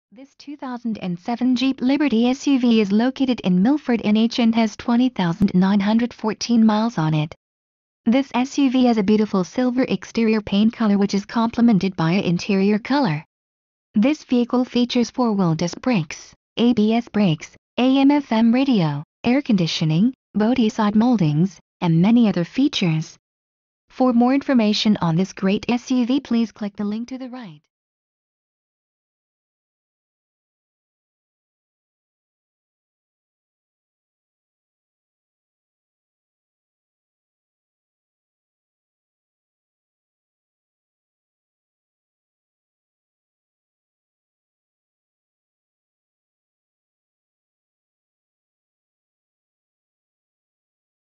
2007 Jeep Liberty Nashua Manchester Keene NH
Contemporary Chrysler Dodge

Price: $ 18,468
Mileage: 20,914
Trim: Limited Edition
Body Style: SUV
Ext. Color: Silver
Int. Color:
Engine: V-6cyl
Drive Type:
Fuel Type:
Doors: 4 doo
Stock No.: 2037-1
VIN: 1J4GL58K87W530114